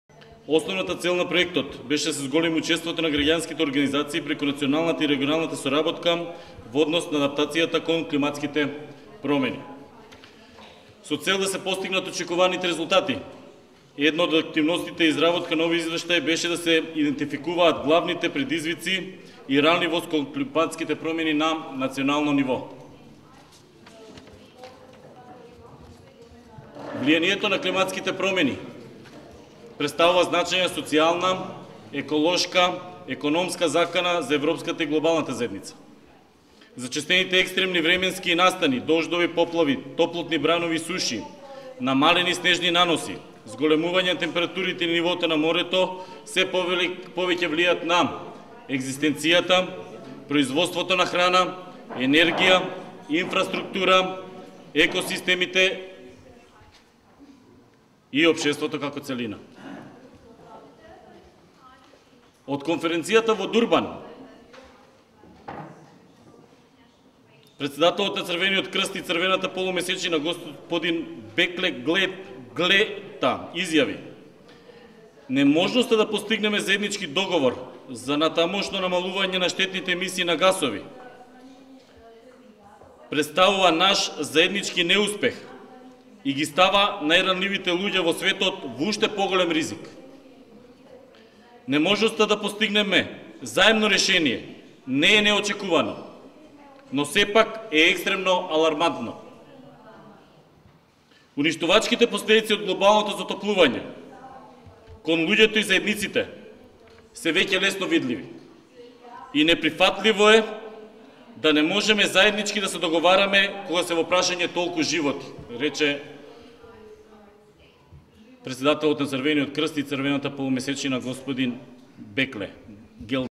Macedonia Climate Vulnerabilty Assessment report: part of presentation at Sutomore, June 2012
Report writer Aleksander Glavinov presented the findings of the Macedonian Climate Vulnerabilty Assessment report in the conference. Please note: due to a technical problem we can only provide you a part of the presentation.

Background: In the "SEE Forum on Climate Change Adaptation" conference in Sutomore, Montenegro (June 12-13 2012) the regional CVA synthesis report was presented. All the report writes also presented their national reports.